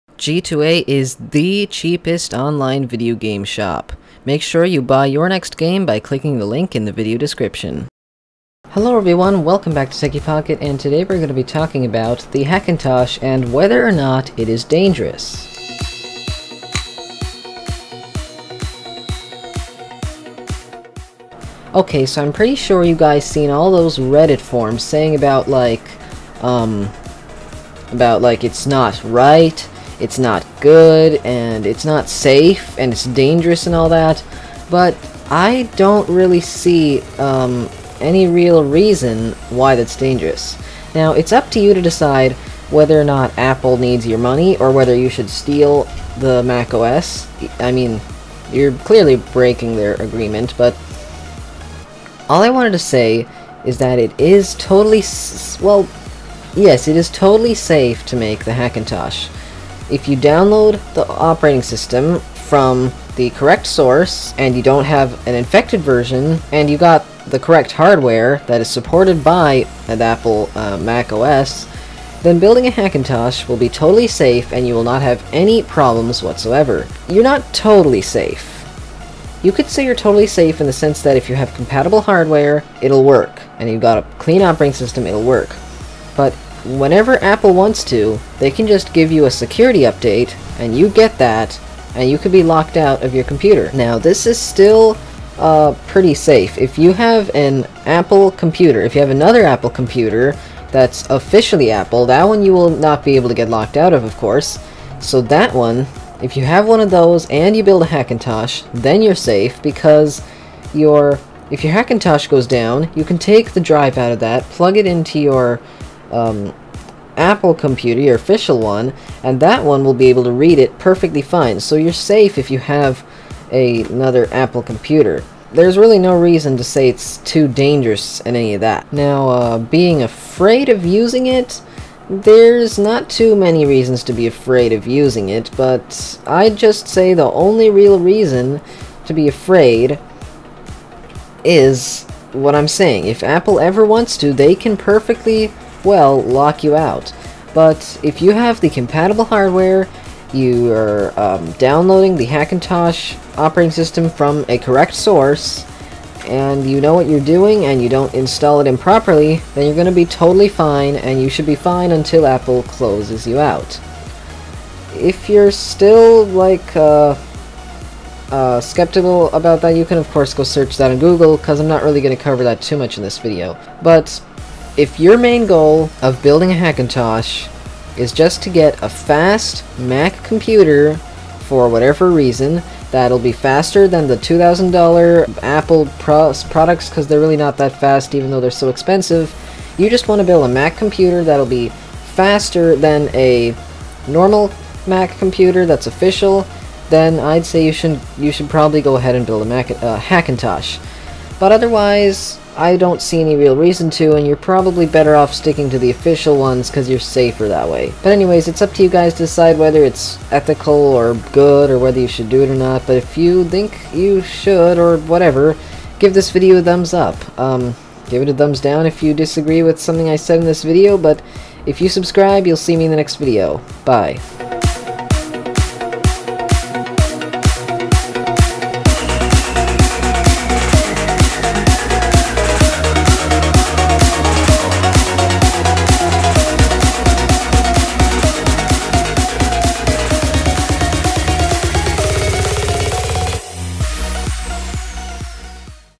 Is a Hackintosh Really All That Dangerous?
Is the Hackintosh really that dangerous? Well in this video I help you decide on your own :)